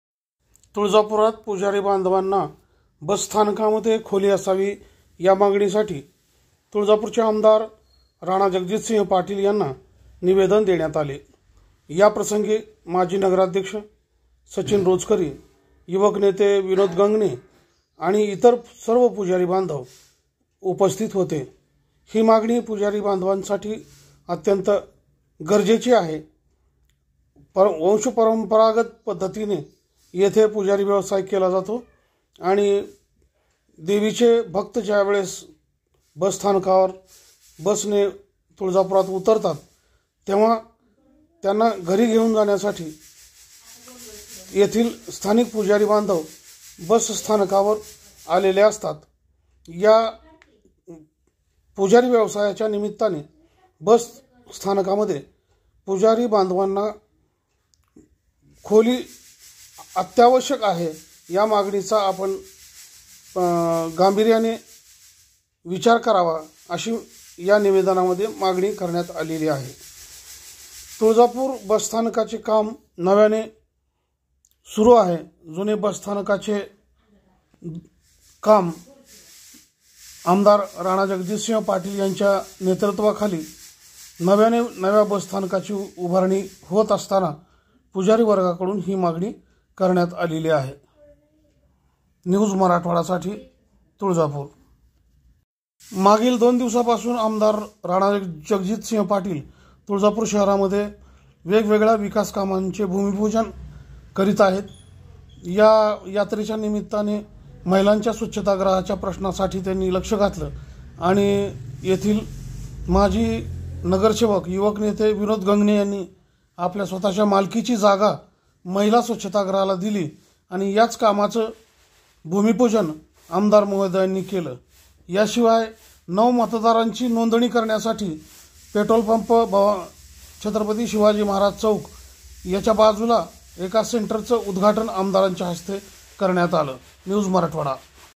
बसस्थानकामध्ये पुजाऱ्यासाठी आवश्यक पुजारी रूम साठी
आमदारांना राणा जगजितसिंह पाटील यांना तुळजापूर येथील पुजारी बांधवांचे निवेदन बस स्थानकाच्या उभारणीमध्ये तुझ्यासाठी असावी या मागणीसाठी निवेदन साद